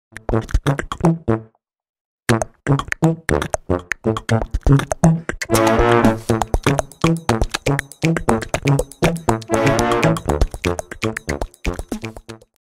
Hartz
A test I made using the free 5-second demo from the NFB's McLaren's Workshop App (dedicated to Norman McLaren).